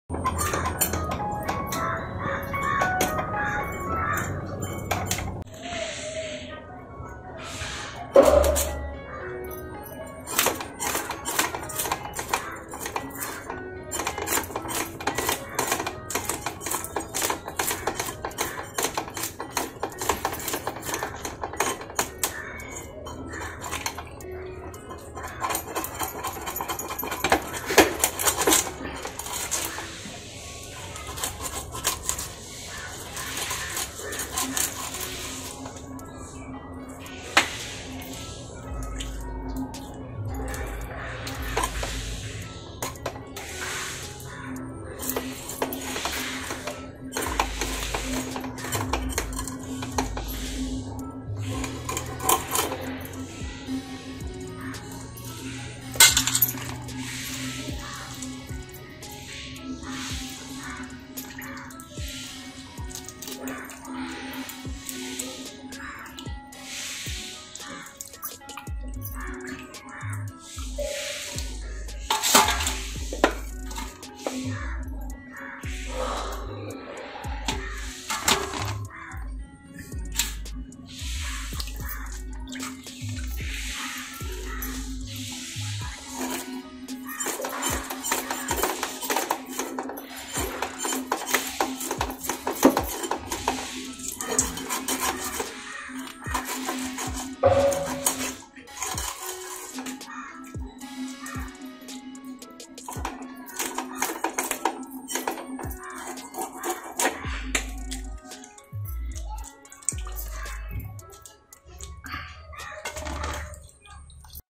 How to Cut and Clean Sardines
In this video we have shown how you can cut and clean sardine fish before it is cooked. Those who don't know the proper way to cut and clean sardines, they can watch this video and can become an expert in cleaning sardine fish. Sardines smells very fishy, but if you clean it properly then the smell can be reduced at some extend and you can cook sardines without smell. Before preparing any dish of Sardines, it is highly recommended to clean the Sardine Fish thoroughly.

Products used in our Kitchen
👉🏼 Pigeon Gas Stove
👉🏼 Prestige Kadai
👉🏼 Mixer Grinder